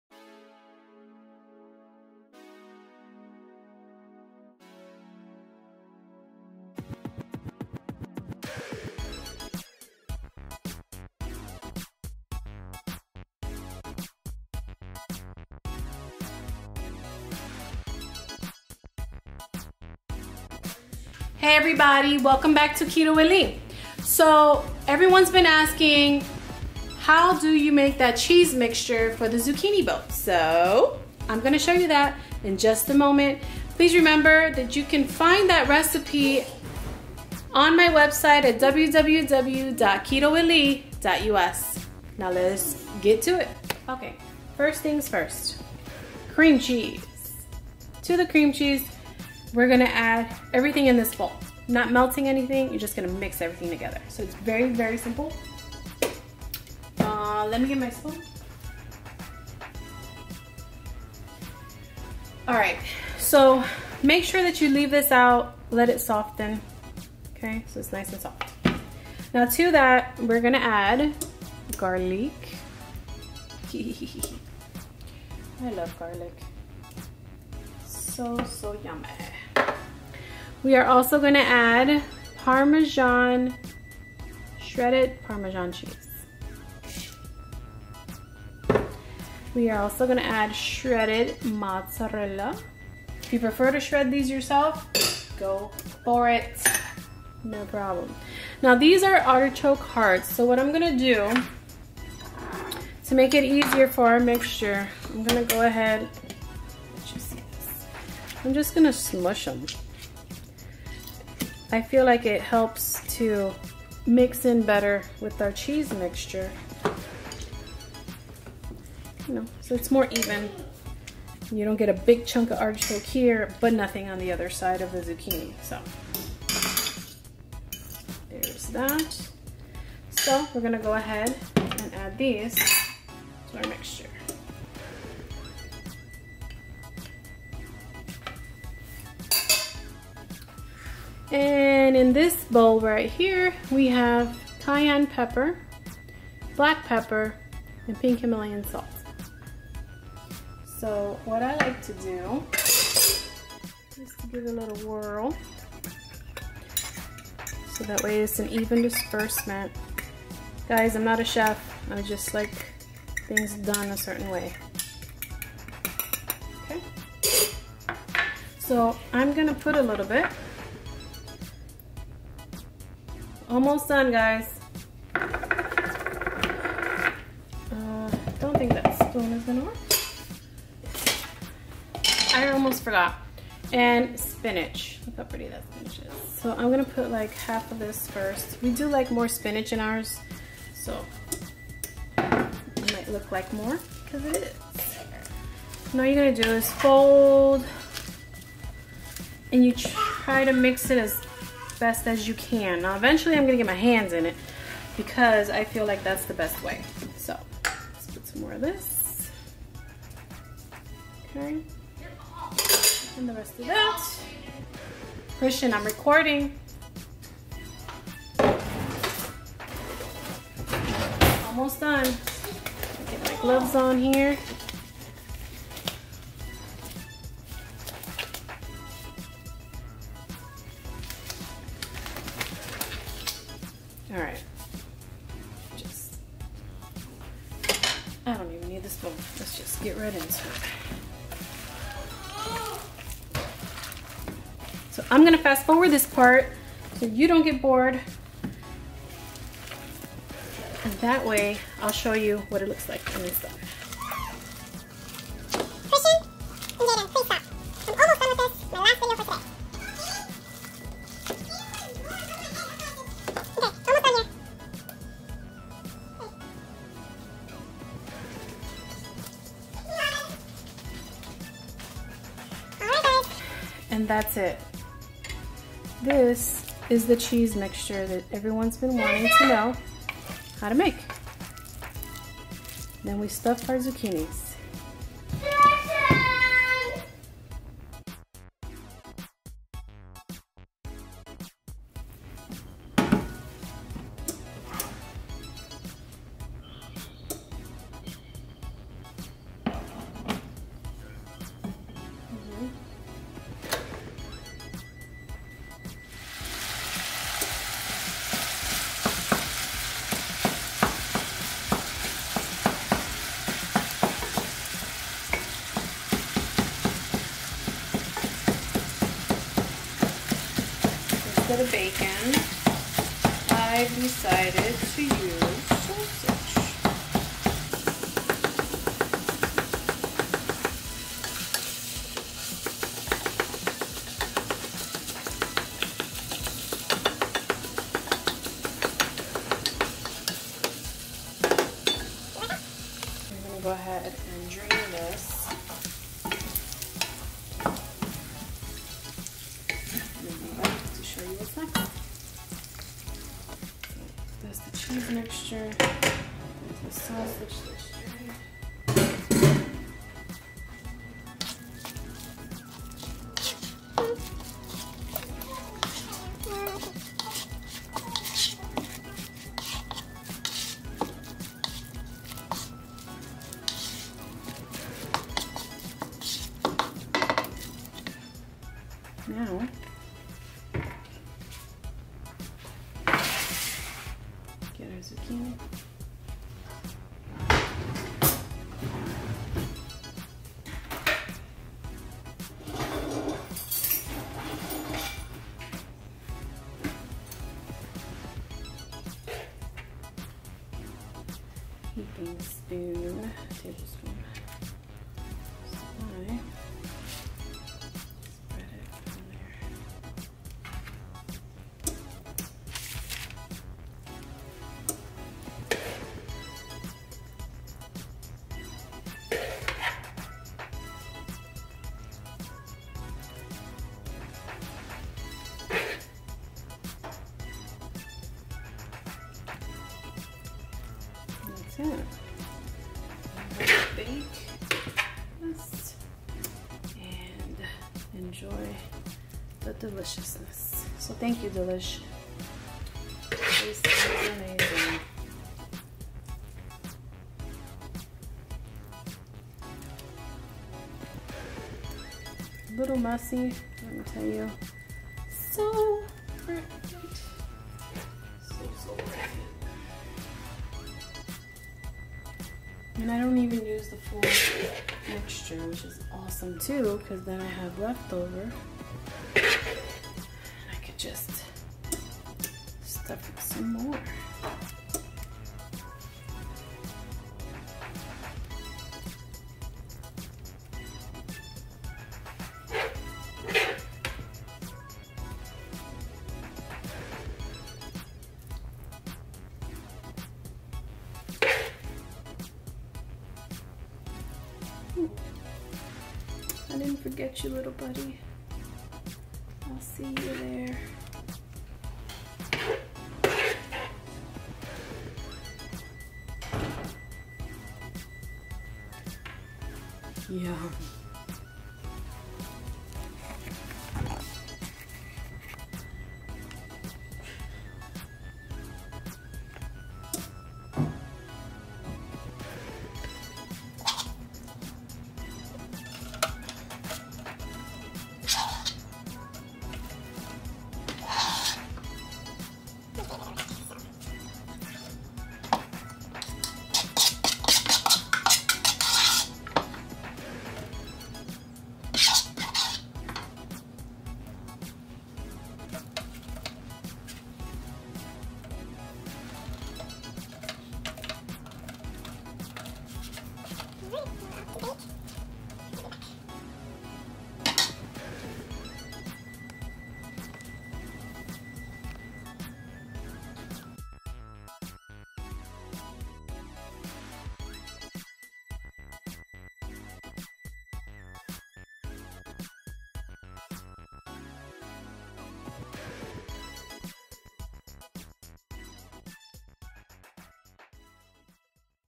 ZUCCHINI BOATS - Low Carb | KETO Friendly Diet Recipe
I put my twist on this DELISH Recipe!

Written recipe:
Ingredients:
-3 –Medium, Zucchini’s
-8 ounces –Cream Cheese, softened
-1/2 cup –Marinated Artichoke Hearts
-1 cup –Fresh / Frozen Spinach
-1 cup –Shredded Mozzarella Cheese
-1/2 cup –Shredded Parmesan Cheese
-1 teaspoon –minced garlic
-1/4 teaspoon –cayenne pepper
-1/4 teaspoon –black pepper
-1/4 teaspoon –Pink Himalayan salt

Instructions:
1. Prepare the Cheese Mixture and set aside.
2. Pre-heat oven to 350*F
3. Remove the seedy centers with either a spoon or an ice cream scooper. The scooper works the best for me!
4. Then transfer these onto a parchment lined baking sheet or dish.
5. Now you can bake these at 350*F for 25 to 30 minutes or until the zucchini is tender and the tops are browned

Follow us on: Instagram  @keto_with_lee AND Twitter @ketowithlee